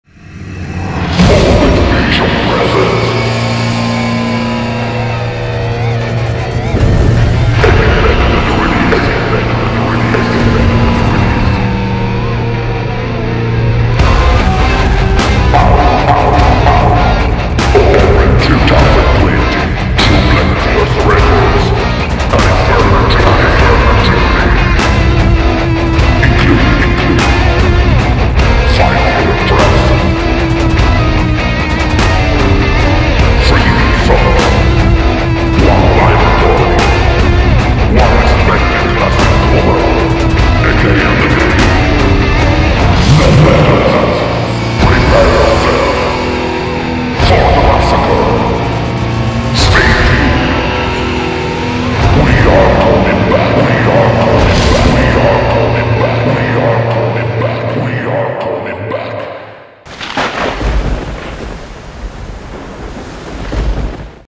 NECRODEATH - New EP out on April, 2 2020 (Official Trailer)
A new Necrodeath release out on April 2, 2020 through Black Tears Records.
An infernal journey, including 5 killer tracks:
- 3 new songs
- 1 live recording
- 1 unexpected classic cover.
Prepare yourself for the massacre... we are coming back!